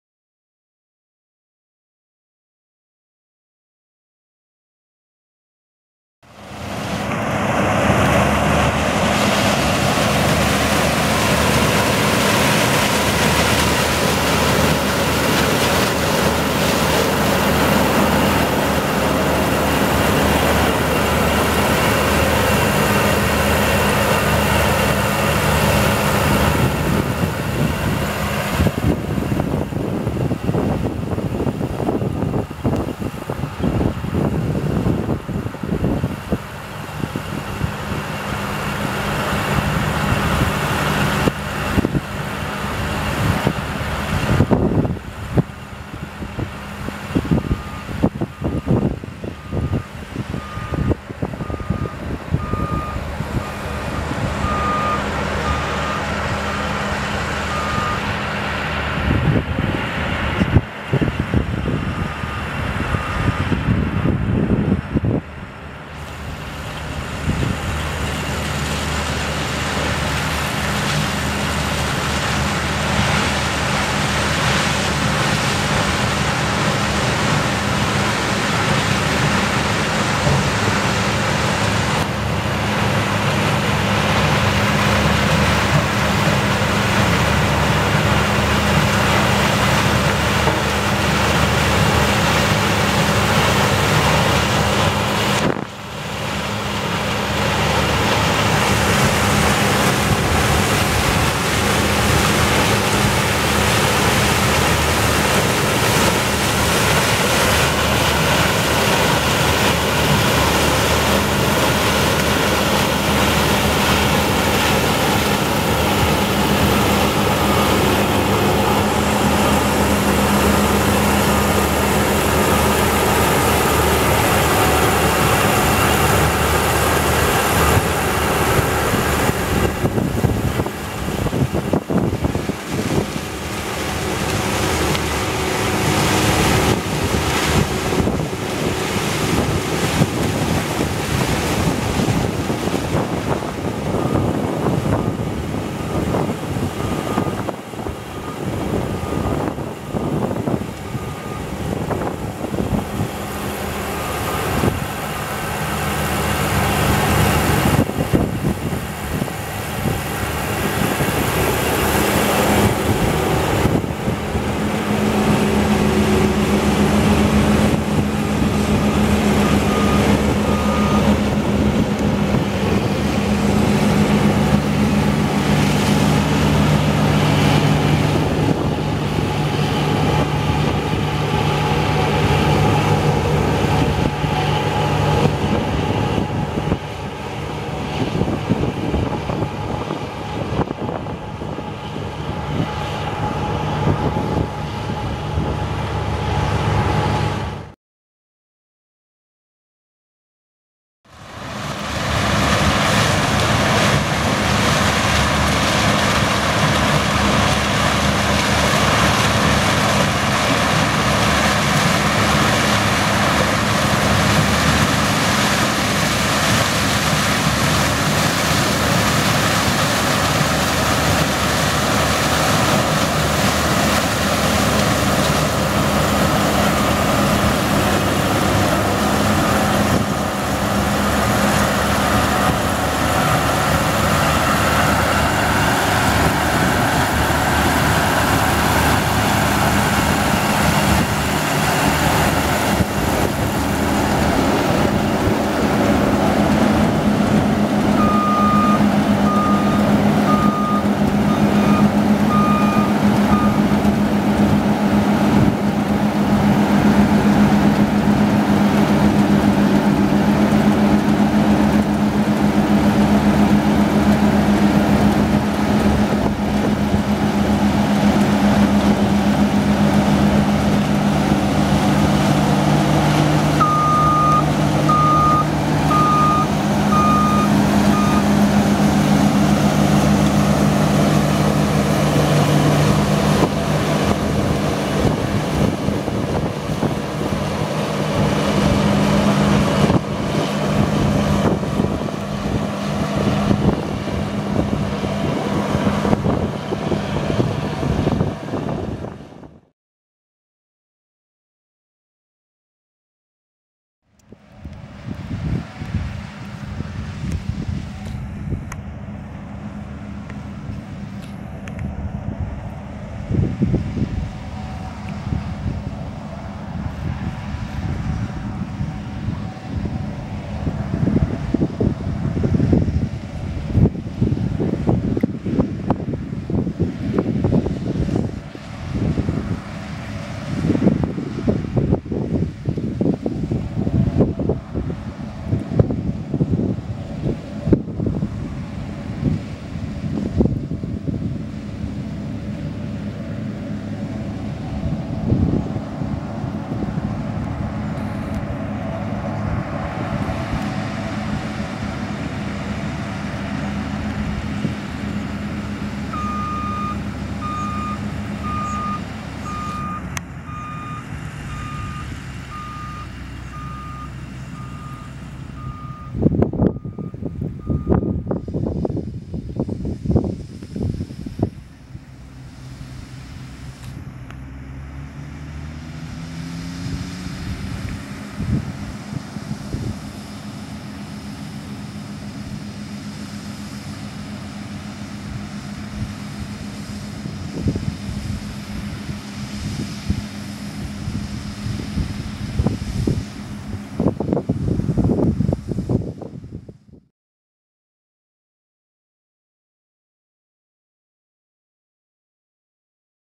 NEW HOLLAND CX7040 mais 2015
Mietitrebbia New Holland CX7040   raccolta mais 03 settembre 2015, Este, PD